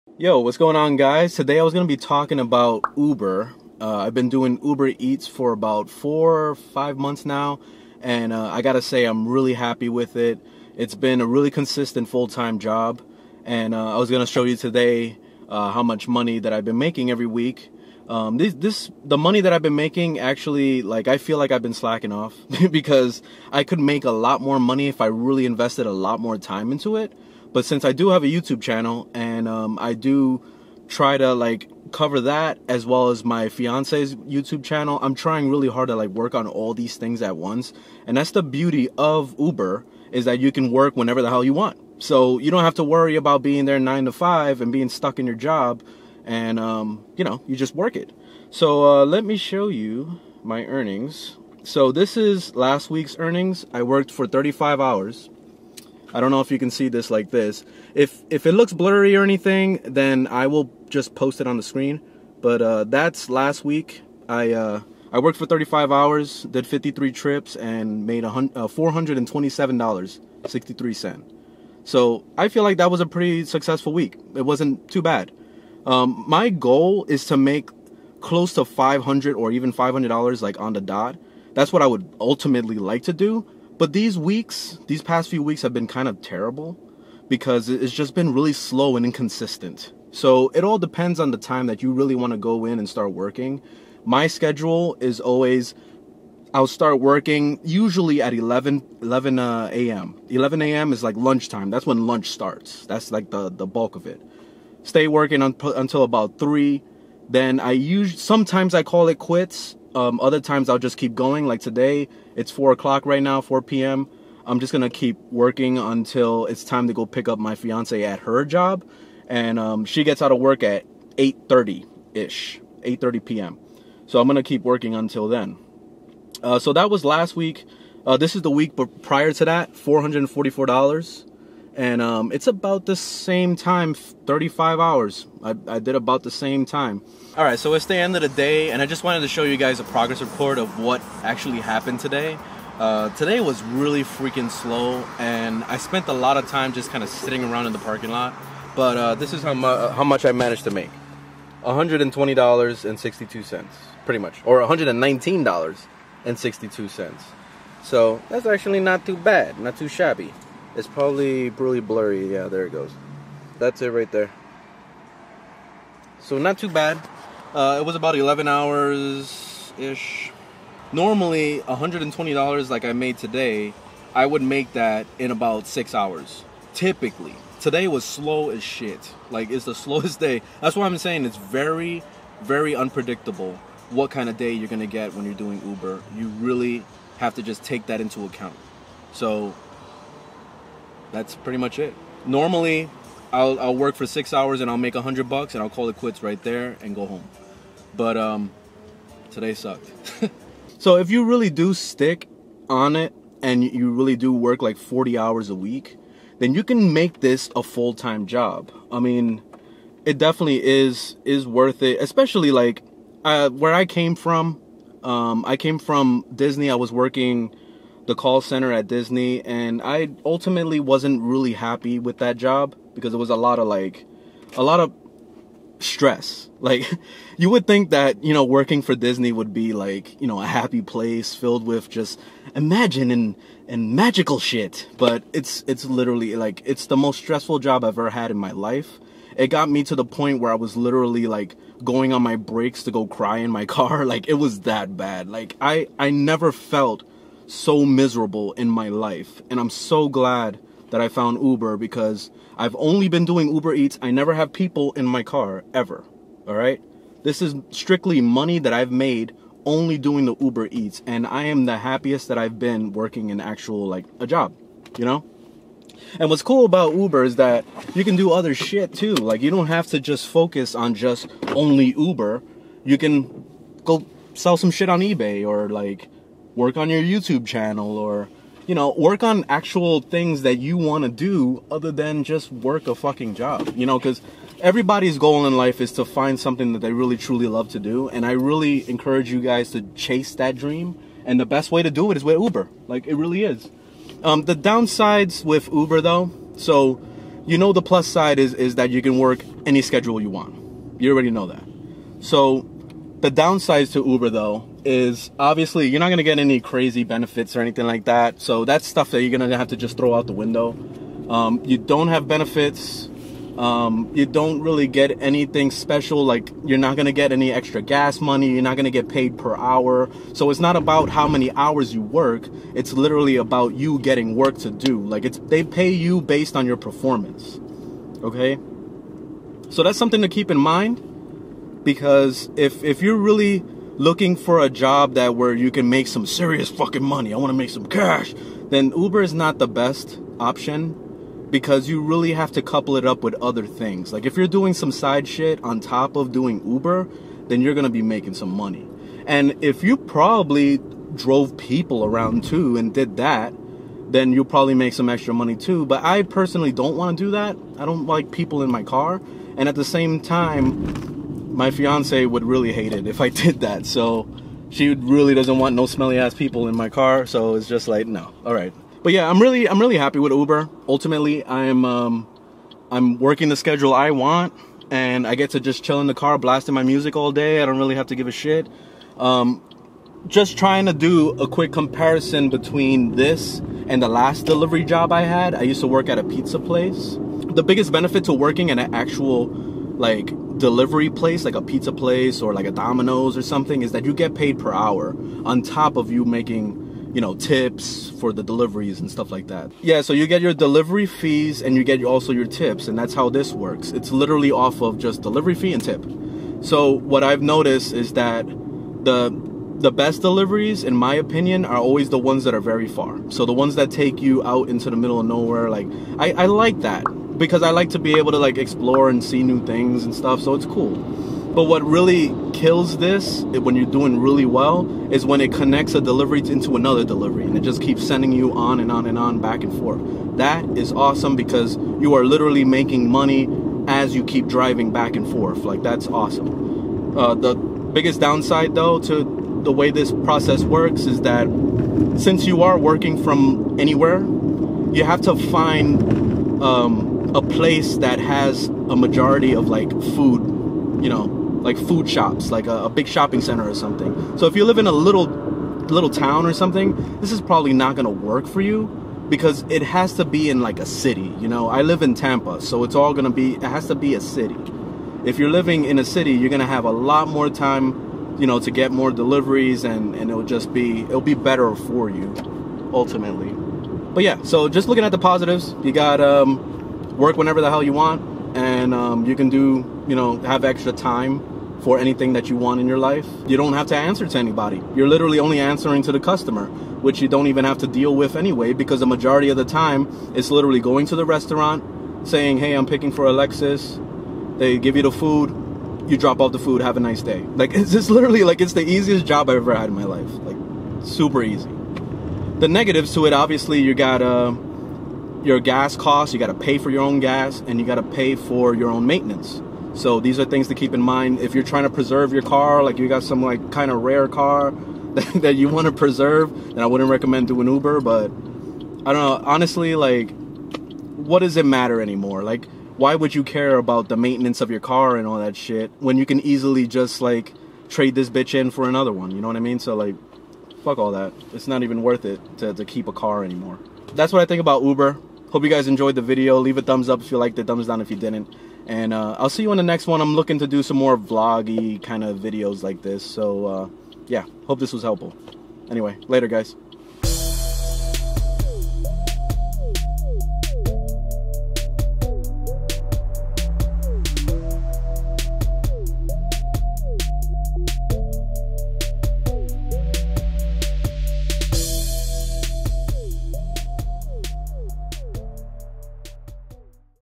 Can Uber Eats Be A Full Time Job?
In this video I show you my earnings report for the last coupe weeks showing how much I made and also share my thoughts on how I feel about Uber as a full time job.